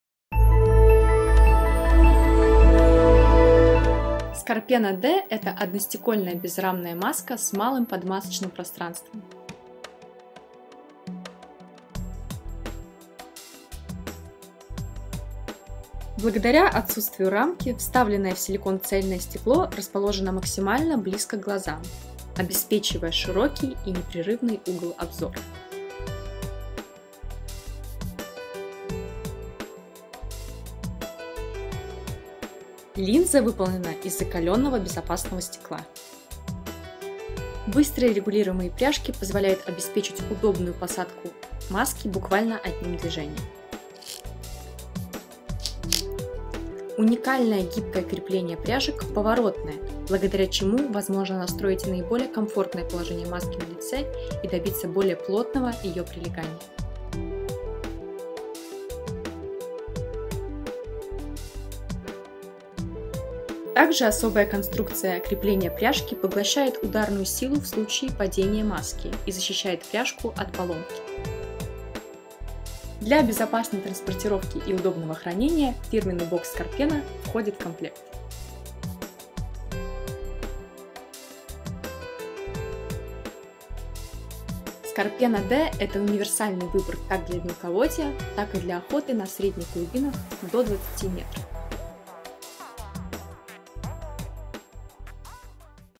Маска SCORPENA D
SCORPENA D – это одностекольная безрамная маска с малым подмасочным пространством.  Смотрите видеопрезентацию модели D из линейки масок народного бренда подводного снаряжения SCORPENA